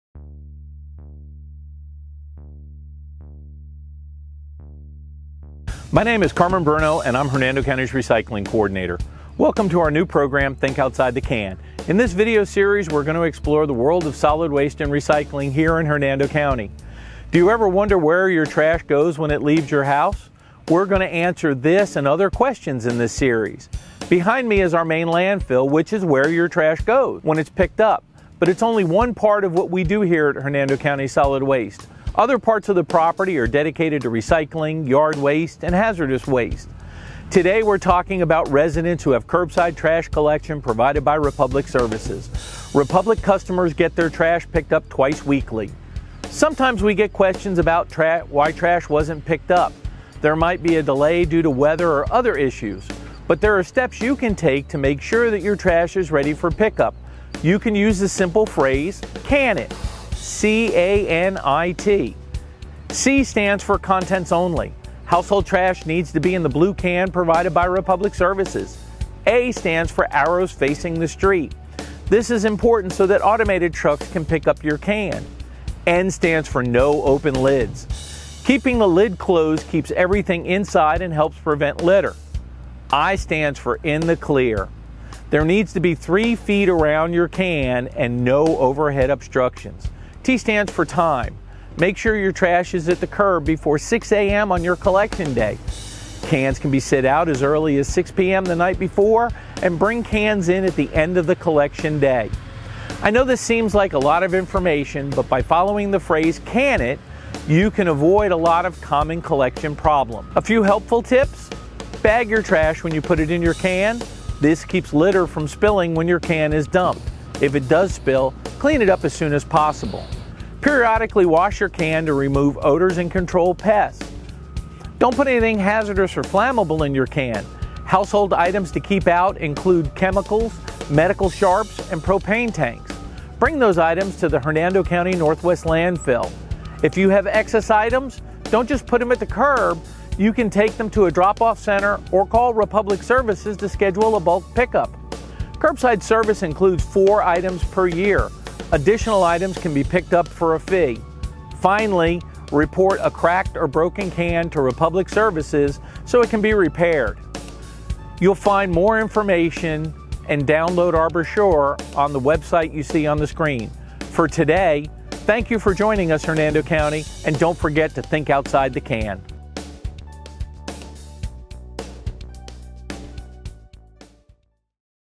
Think Outside the Can - Hernando County Curbside Trash Collection
Meet Carmen Bruno, Hernando County's Recycling Coordinator! Join us in exploring the world of solid waste and recycling in HernandoCounty as Carmen introduces our newest video series, Think Outside The Can.

In this video, we talk about curbside trash collection in our County, performed by Republic Services. Learn the steps you can take to ensure your trash is ready for pick up with the helpful acronym, C.A.N.I.T., and tips on being a good neighbor.

Thank you to Republic Services and their staff for working hard to help keep our neighborhoods clean and HappyInHernando!